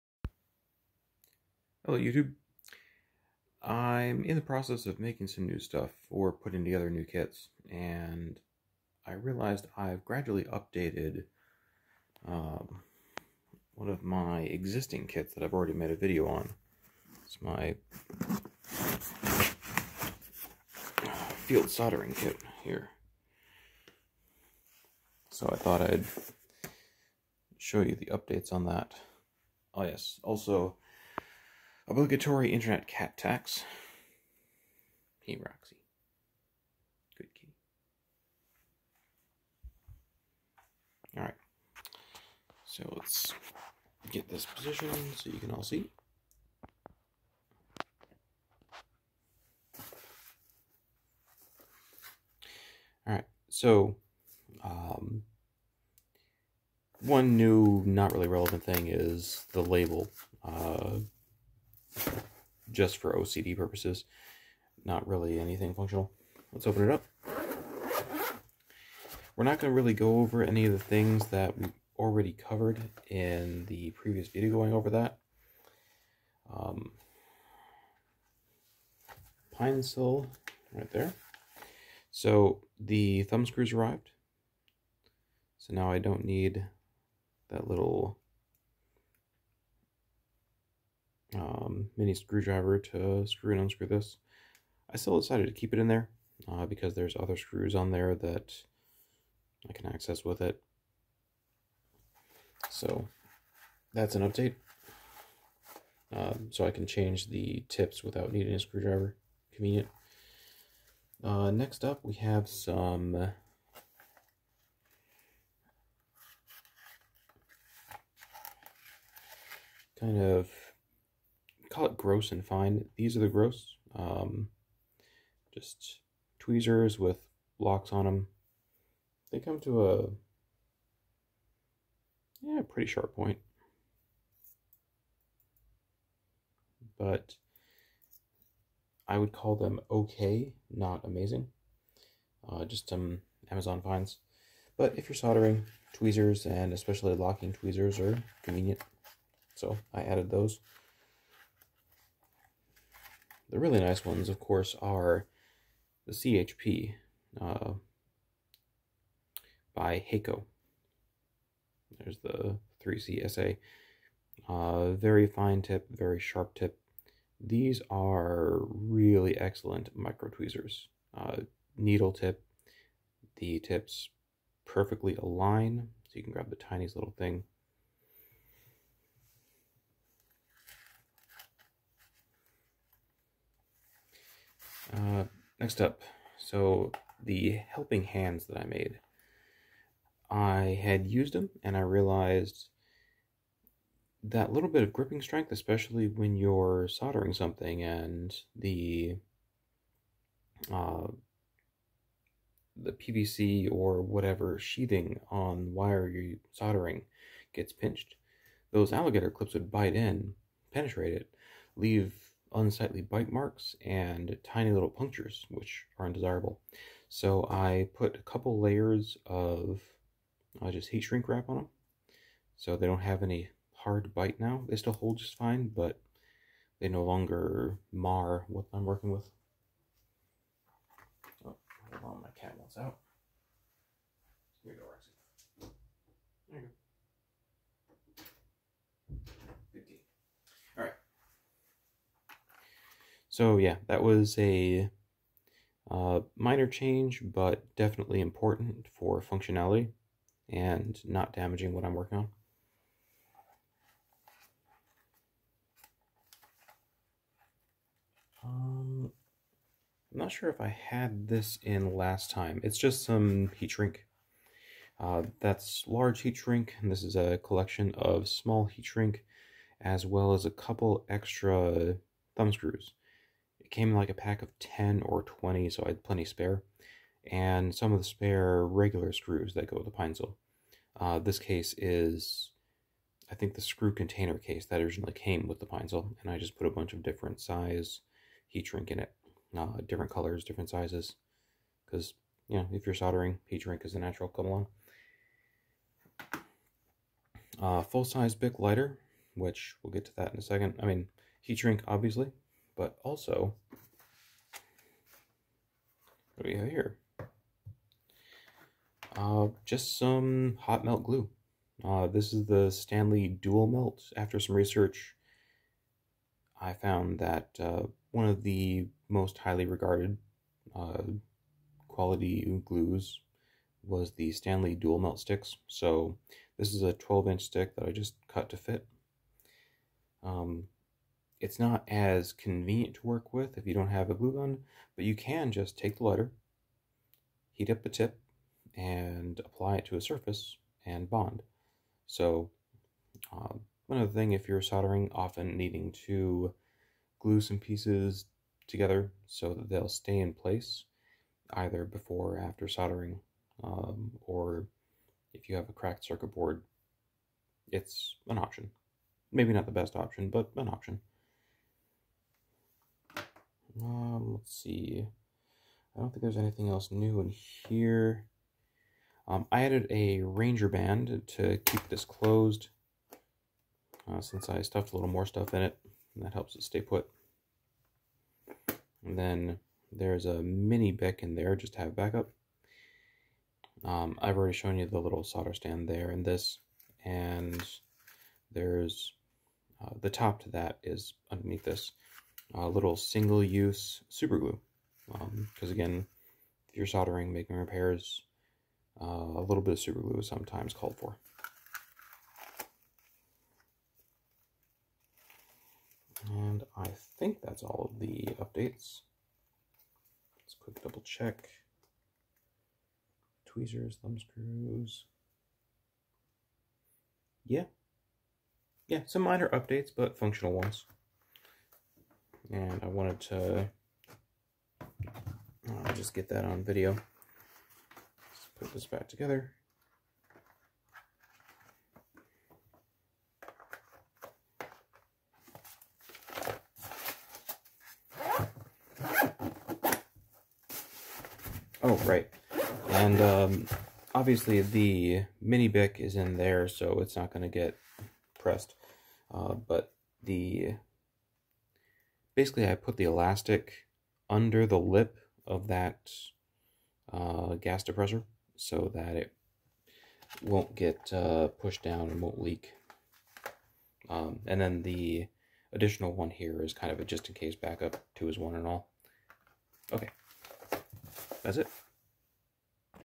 Updates to the Field Soldering Kit (with Pinecil V2).
An update video for my FSK, between a few additions and functional improvements I felt there were enough changes to want to share them.  Enjoy.